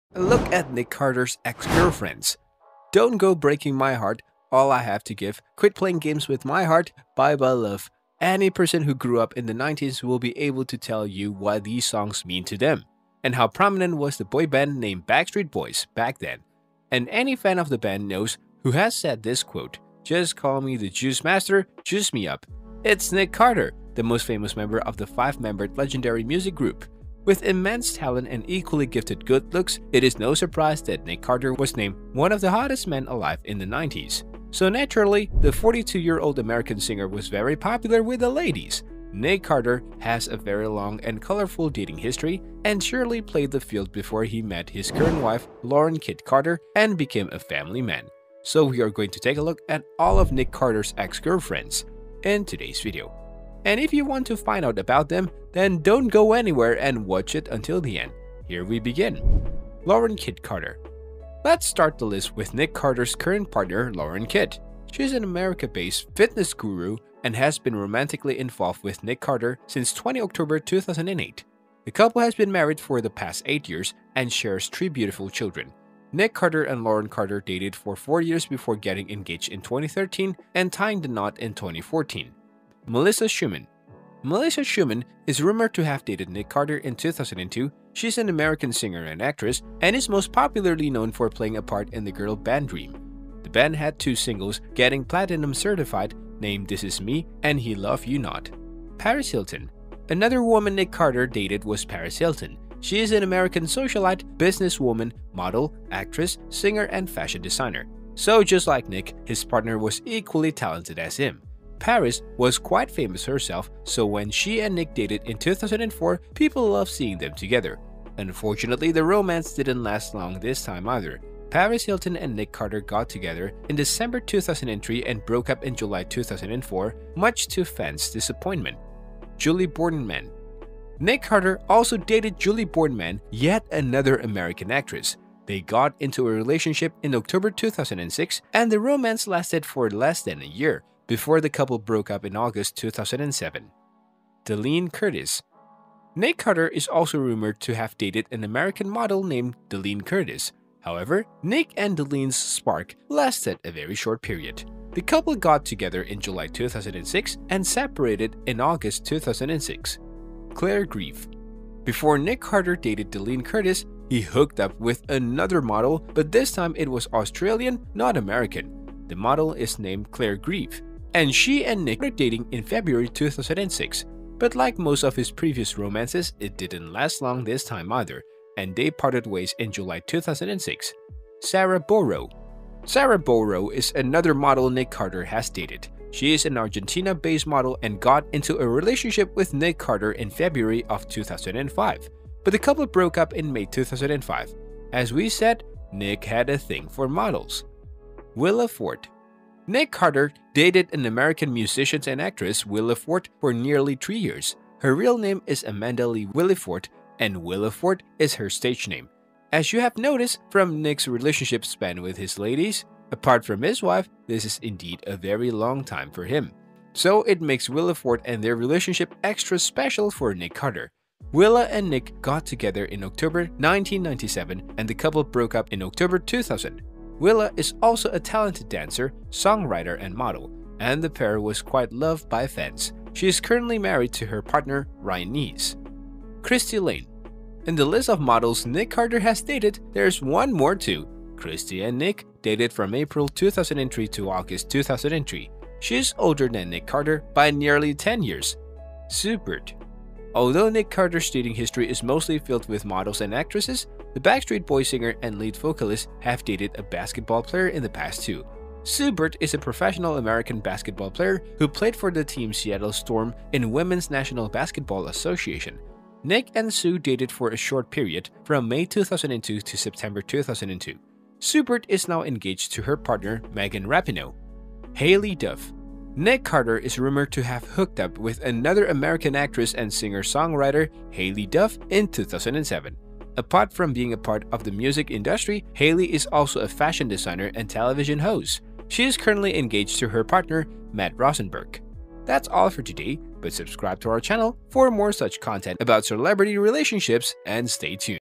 A Look At Nick Carter's EX-GIRLFRIENDS!
Hello there, and welcome to That Happened! Former Backstreet Boys member Nick Carter might be married to Lauren Kitt, but he has surely been romantically linked to several women in the past. So in today's video, we will take a closer look into his dating history.